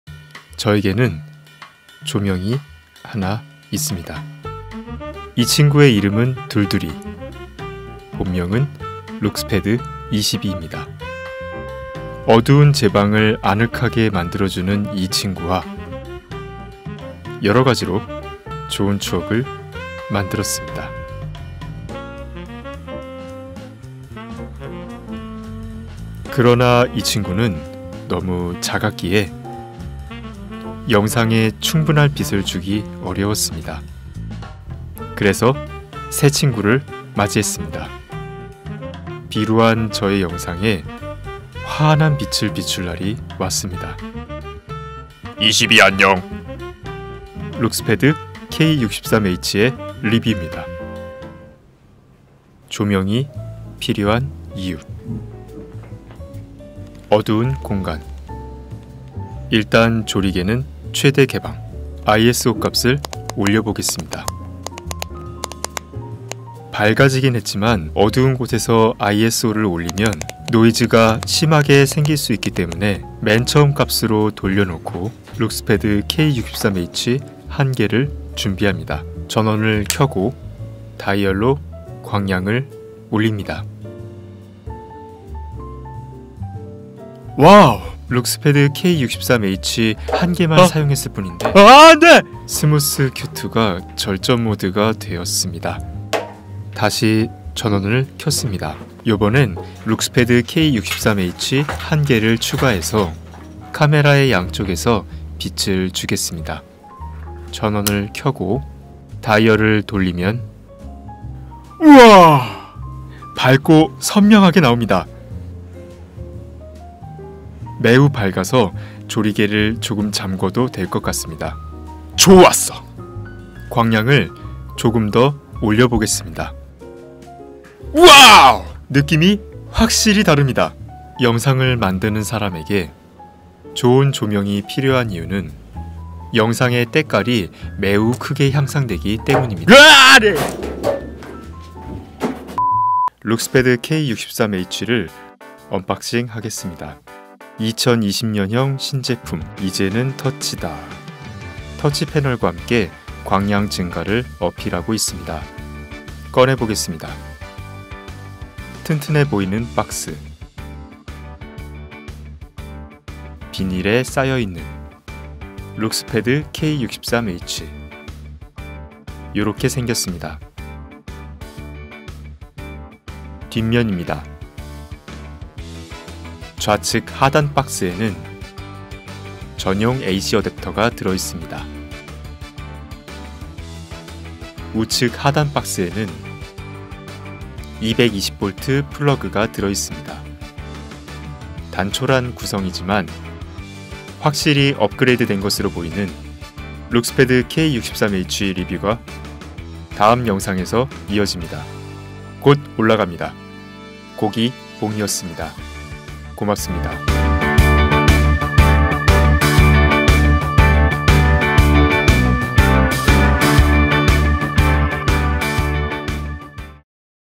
[유쾌한생각]룩스패드 K63H 본격 리뷰! - 언박싱
안녕하세요!
유쾌한생각입니다:)

1인미디어 대표 조명,
룩스패드의 3세대 룩스패드 K63H 입니다!

광량이 더욱 개선되고
전면 터치 패널이 추가된 신상 조명!

자세한 사항은 영상을 참고해주세요!

사용한 제품 : X-T3, X-A7, LUXPAD K63H

카테고리
노하우/스타일